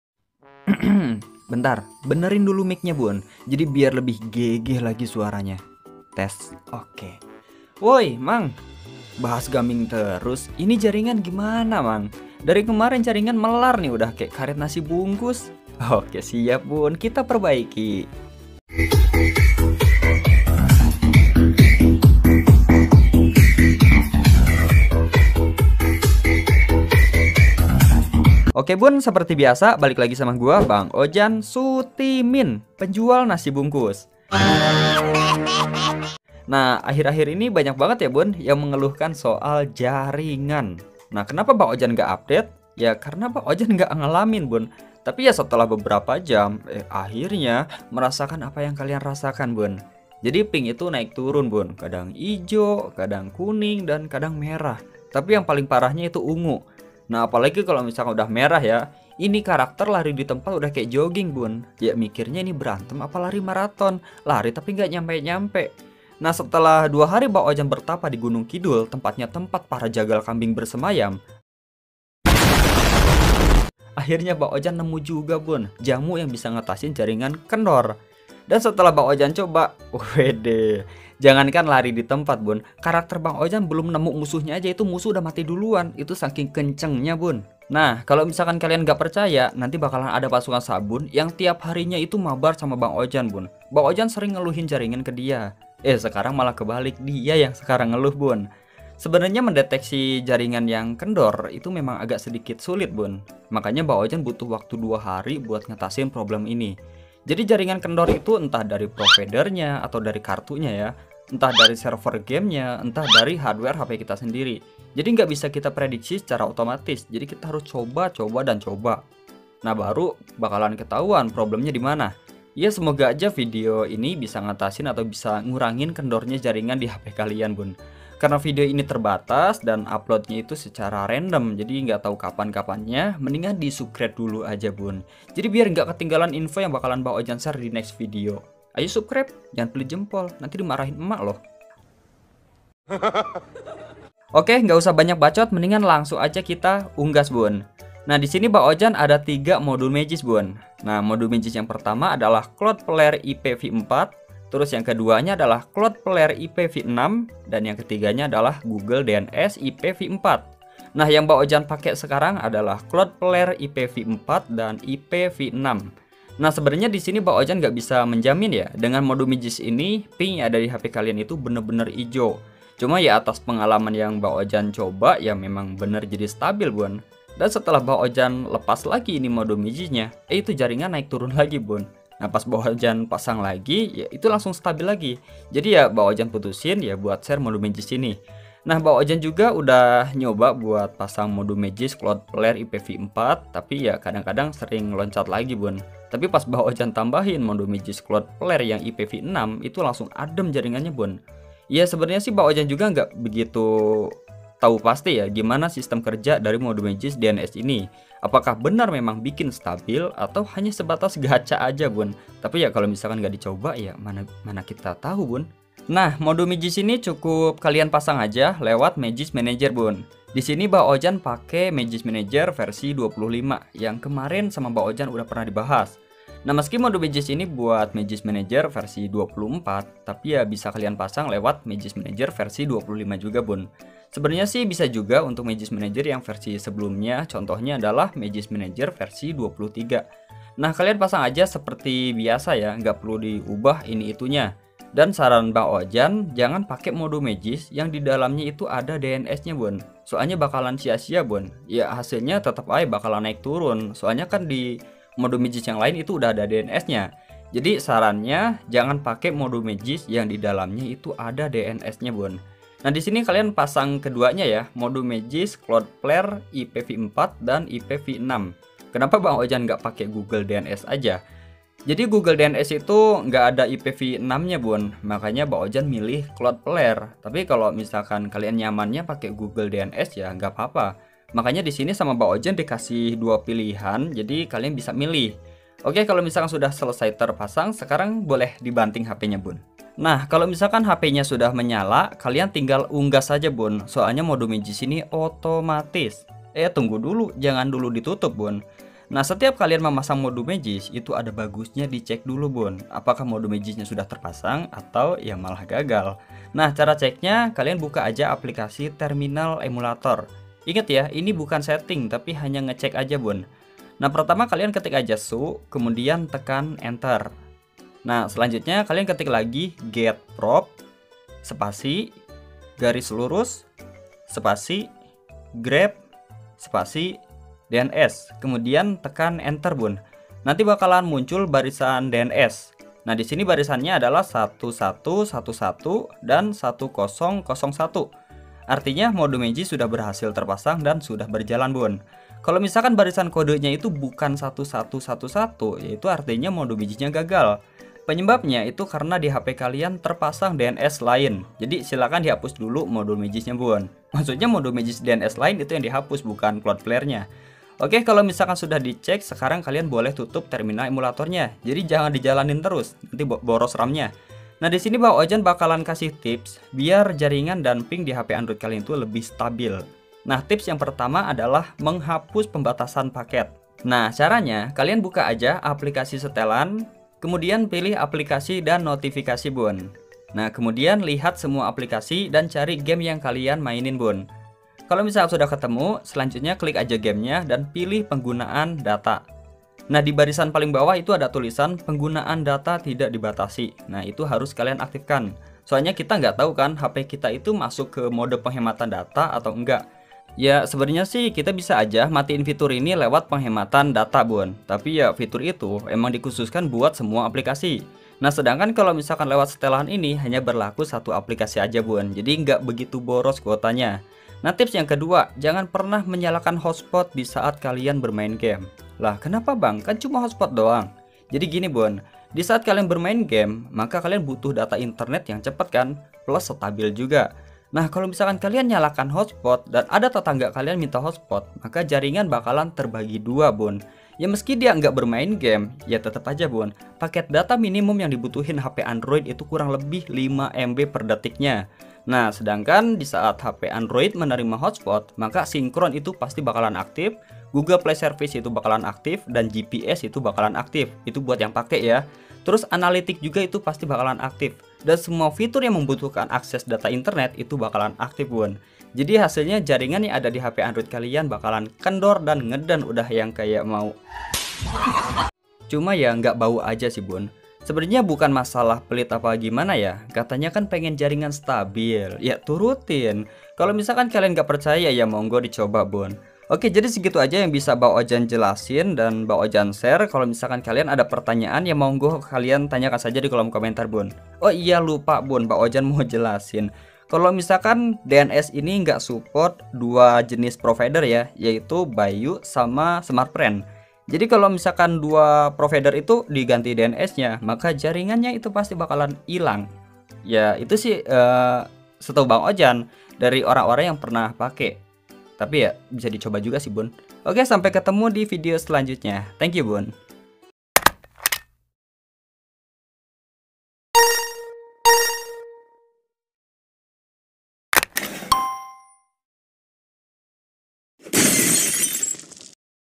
Rahasia Update Baru: Ping Merah Jadi Stabil di Semua Game Android!
NEW TRICK!!! FIX JARINGAN LAMBAT DAN PING MERAH GAME ONLINE ANDROID UPDATE JUNI 2022